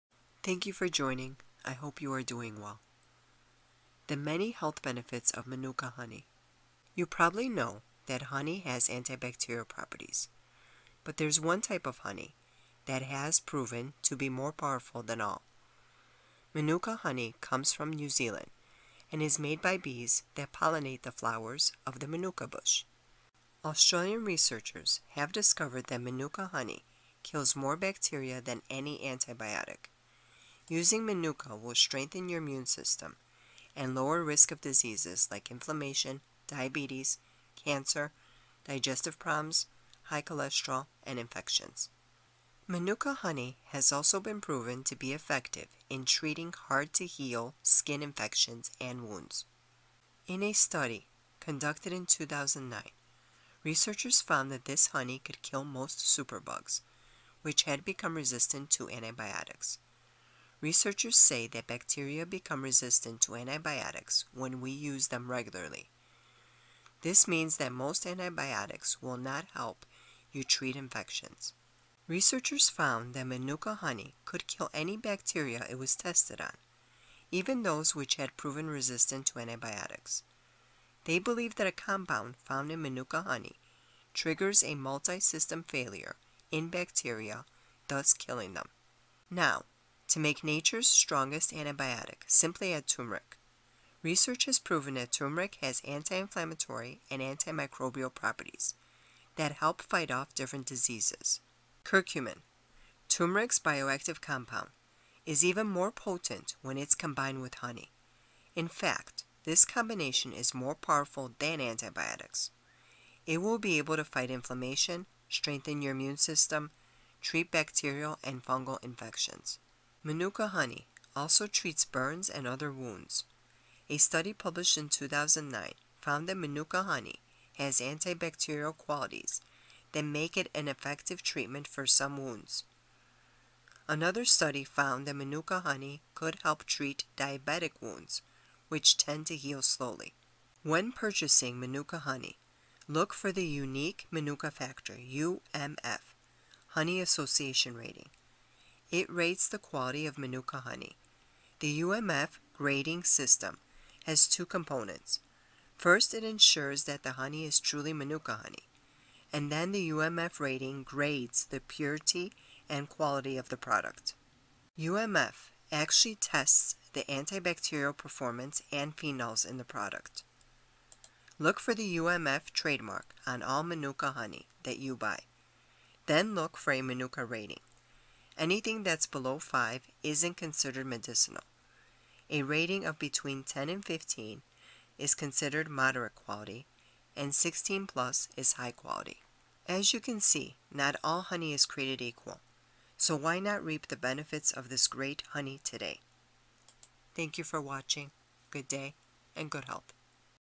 Amazing Benefits of Manuka Honey - Eliminates Resistant Bacteria, Heals Infections & Wounds Fast
You probably know that honey has antibacterial properties. But there’s one type of honey that has proven to be more powerful than all. Manuka honey comes from New Zealand and is made by bees that pollinate the flowers of the manuka bush.  Australian researchers have discovered that Manuka honey kills more bacteria.